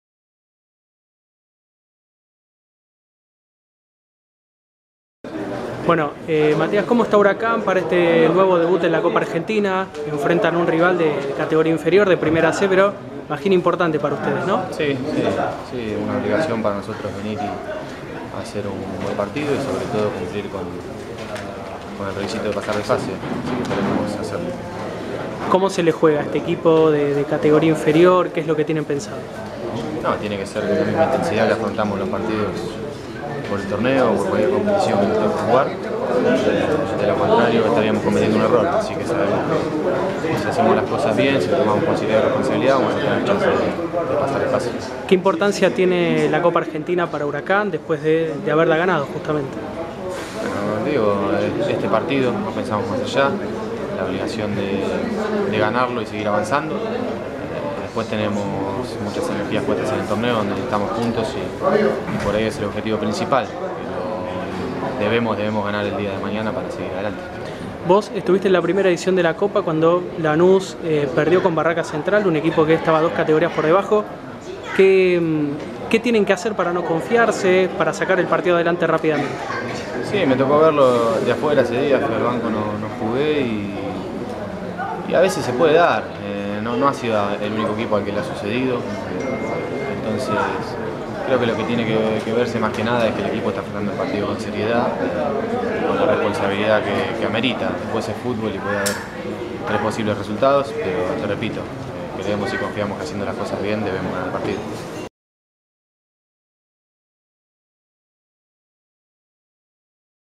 Matías Fritzler - Huracán
El volante del Globo analizó el encuentro de este miércoles ante Defensores Unidos, por los 32avos. de Final.